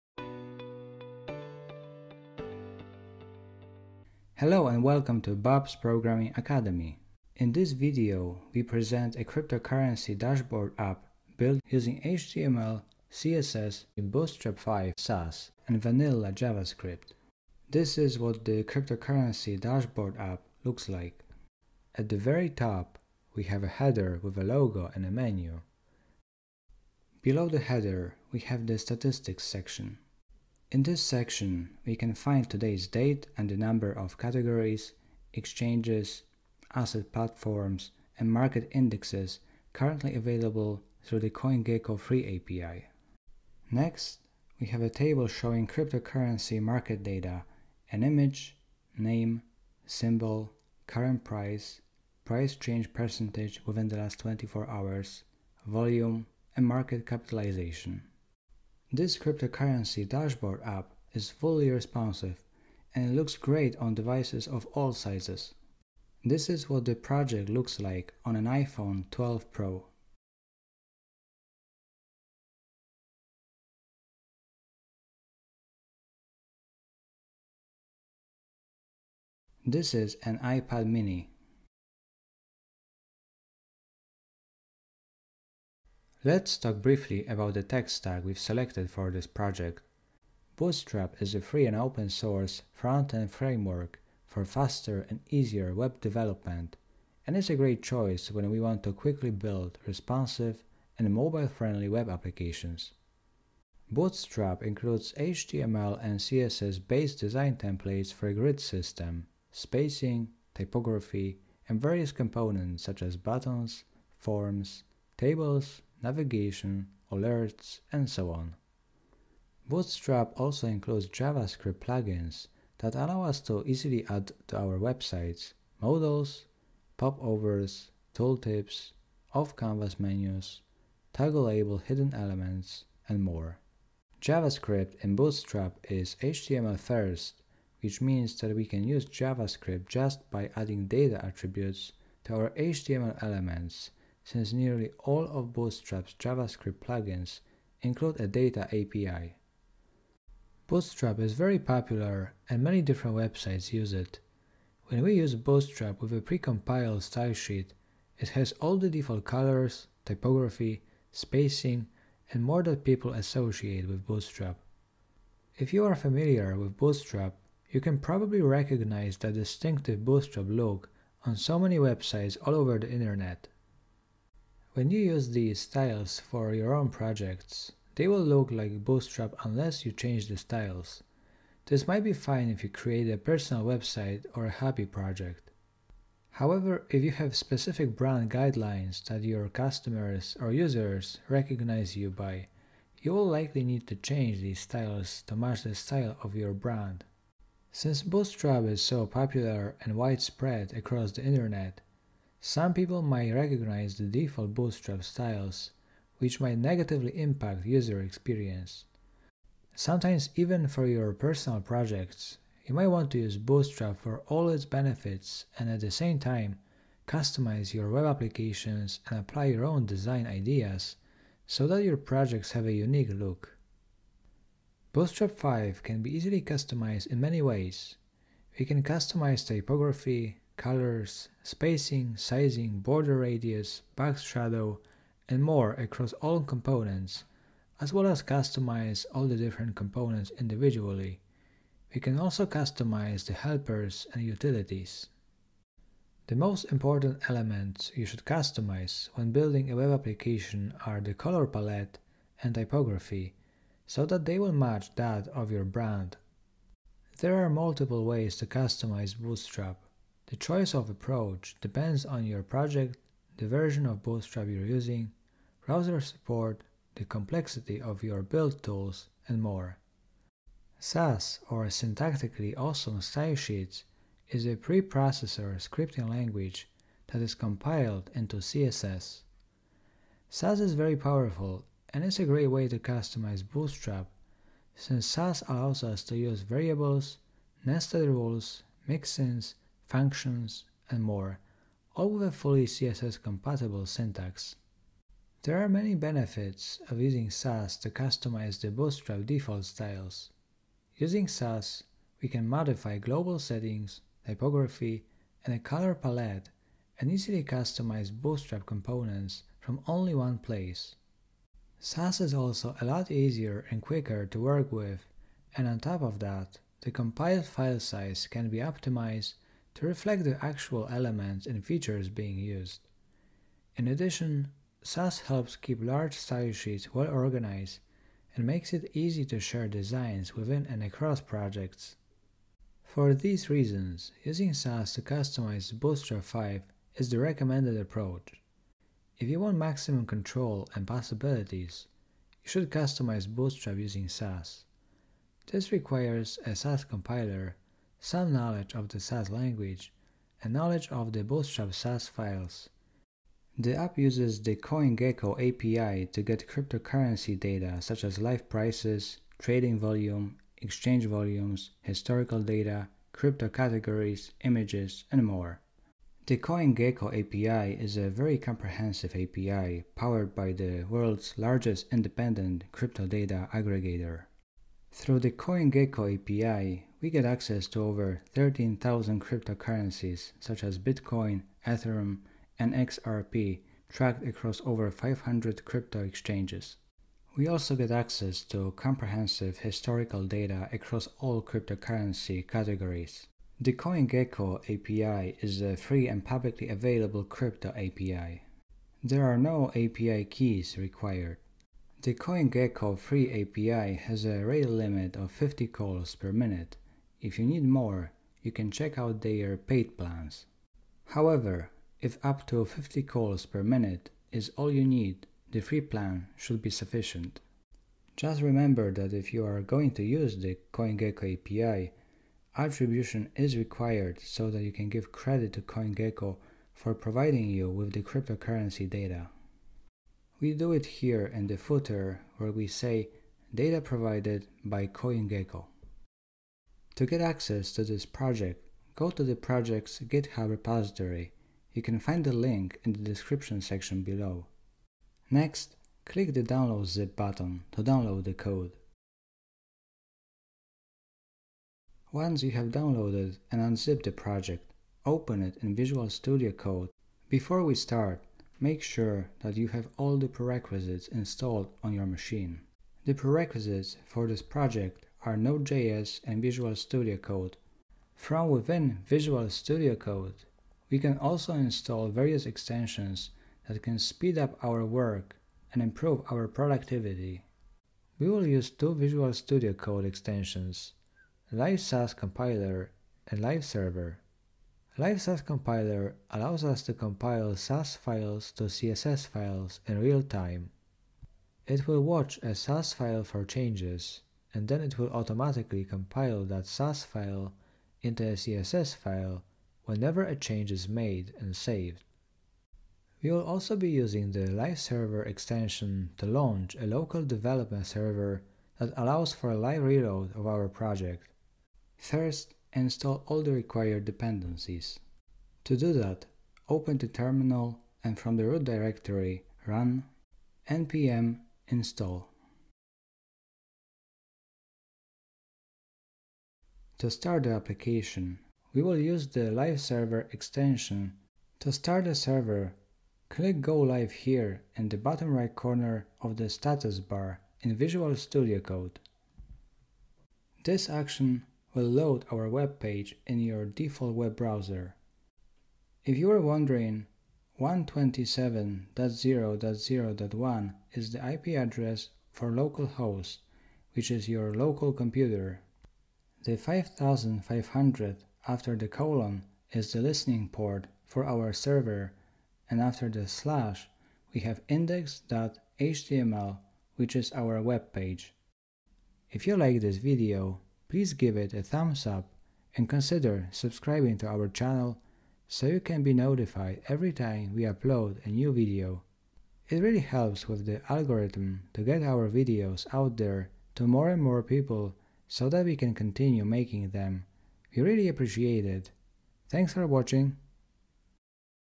Cryptocurrency Dashboard | HTML 5, CSS 3, Bootstrap 5, Sass, and vanilla JavaScript | CoinGecko API
In this video, we present a cryptocurrency dashboard built using HTML 5, CSS 3, Bootstrap 5, Sass, and vanilla JavaScript.The app uses the CoinGecko API to get cryptocurrency data such as live prices, trading volume, exchange volumes, historical data, crypto categories, images, and more.

00:00 Intro
00:17 Project Overview
00:58 Responsive Design
01:26 About Bootstrap
04:30 About Sass
05:53 About CoinGecko API
07:16 Installation
09:28 Outro